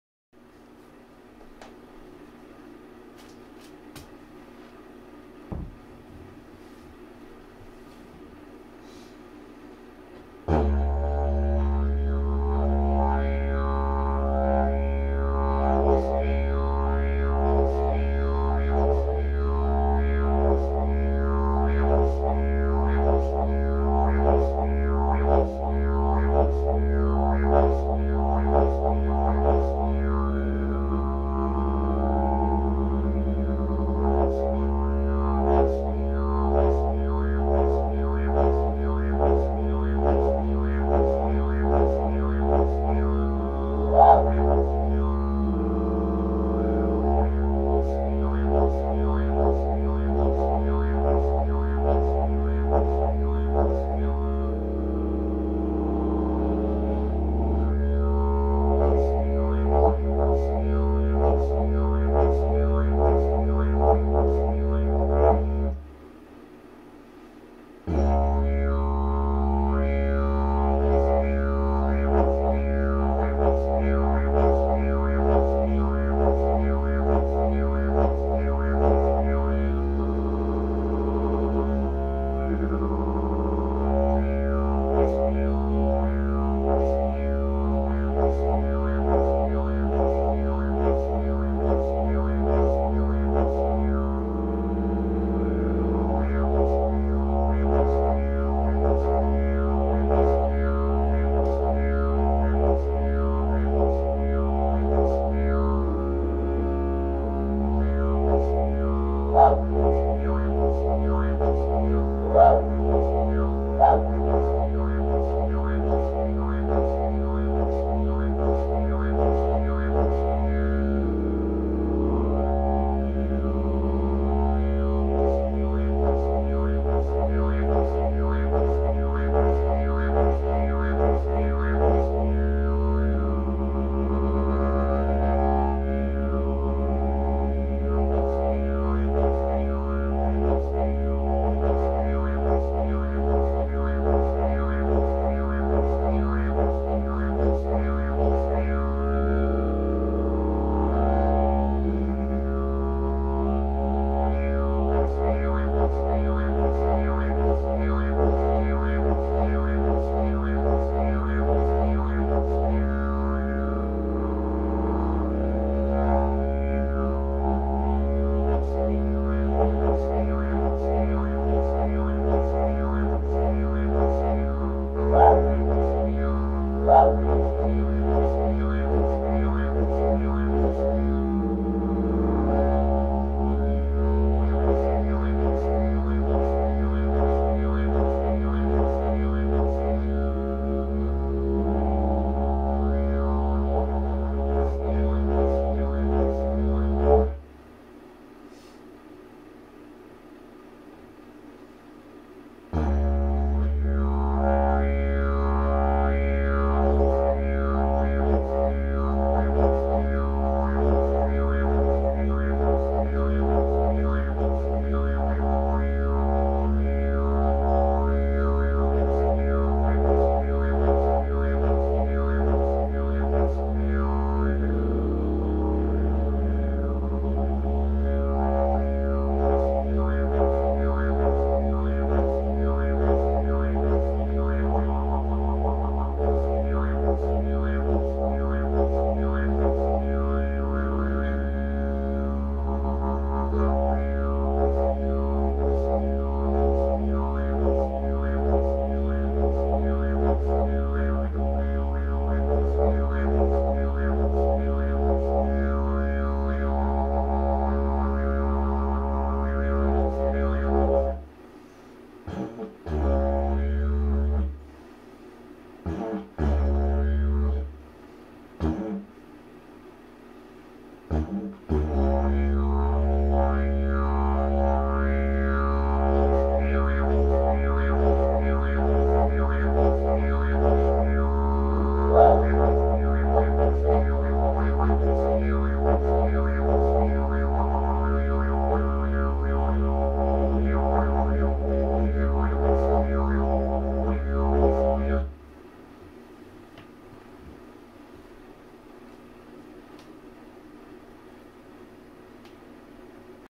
My Didgeridoo Journey (Day 302)
Quietly trying to accomplish making a video of me practicing playing my Didgeridoo everyday for a year. I've had it ever since my family and I lived in Australia for a year when I was 11 and have never put in the practice I have wanted to.
This is to help keep me accountable in my practice aswell as to document my journey

@DidgJourney on twitter